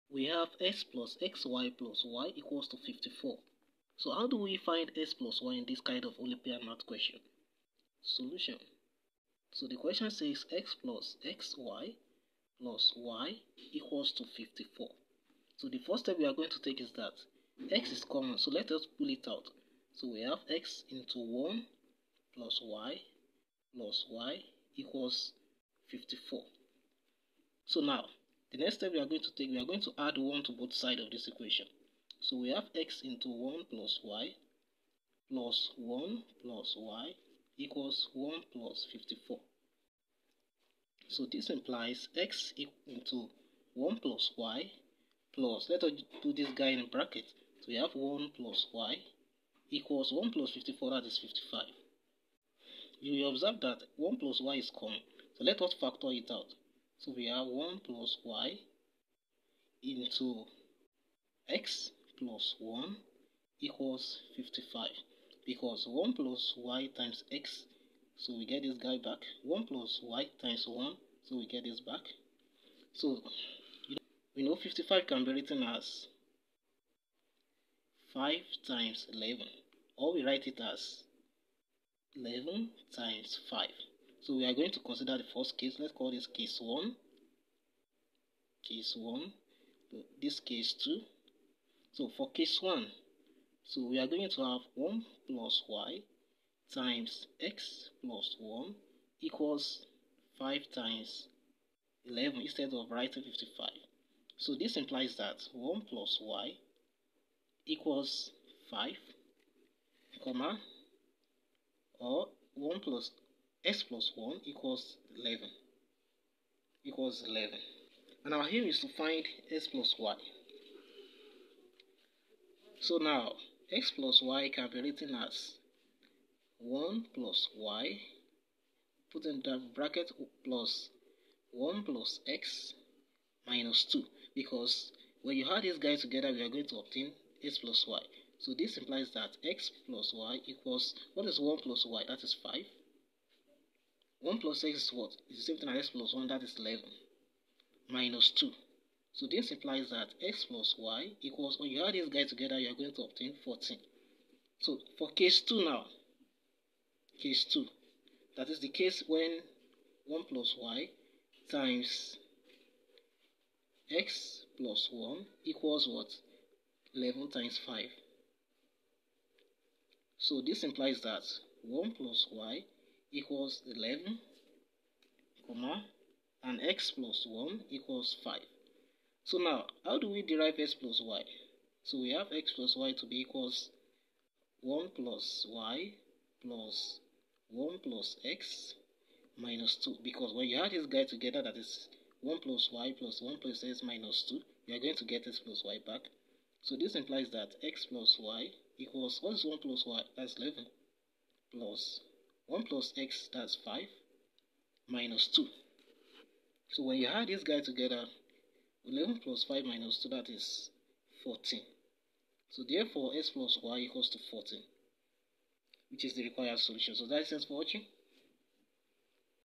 Olympiad Math Question | Solving Tricky Olympiad Math Question
In this video you will learn how to solve an olympiad math question or you will learn how to solve a tricky olympiad math question. you should know this trick!